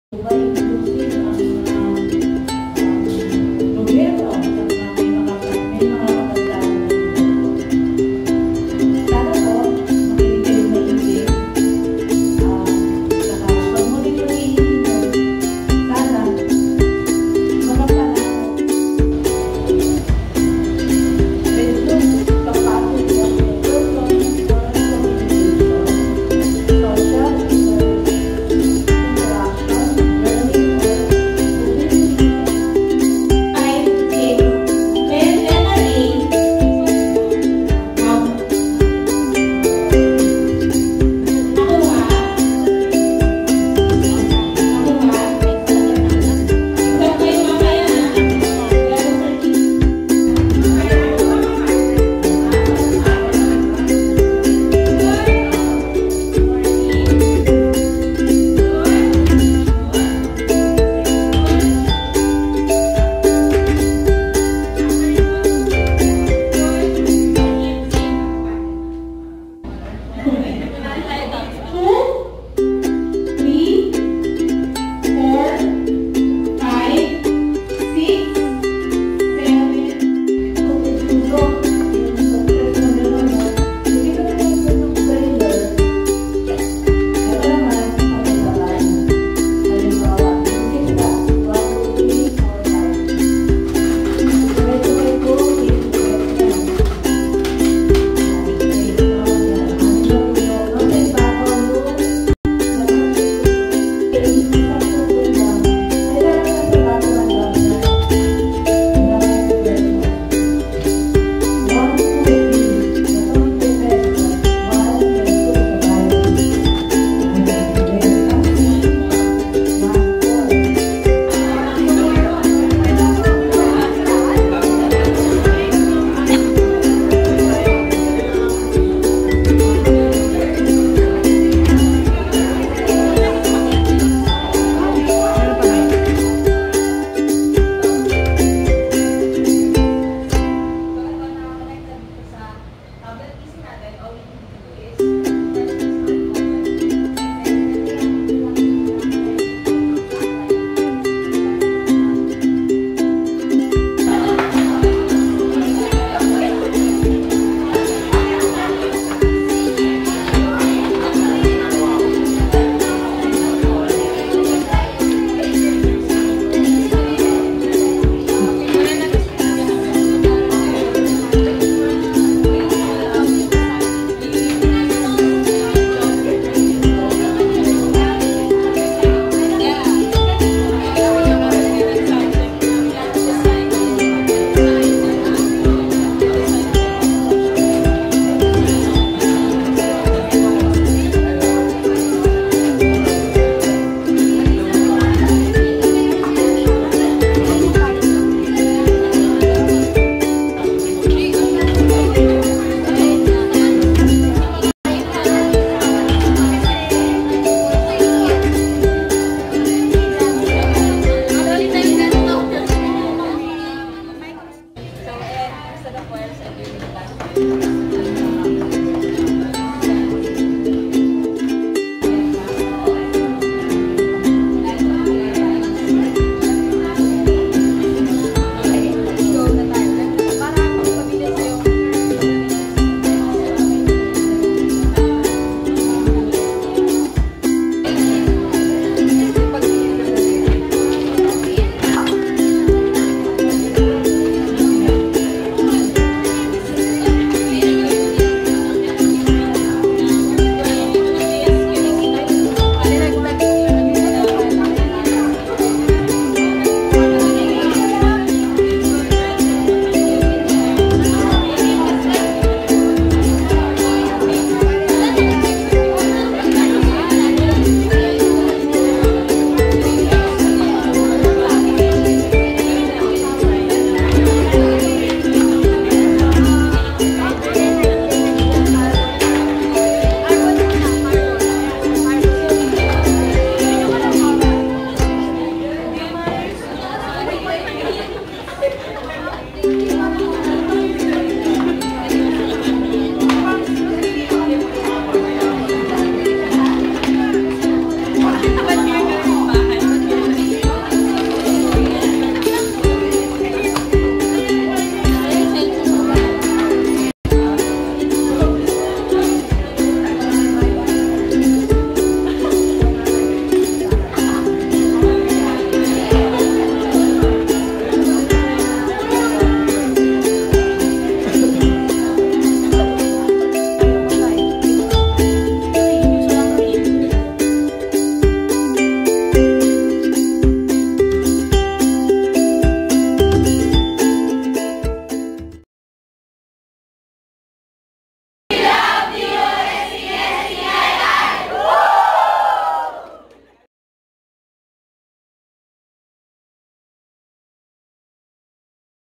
DOST STII Library National Book Week Celebration 2019 Day 3
Highlights of the DOST-STII Library National Book Week Celebration conducted on 7 November 2019 at DOST-STII Library.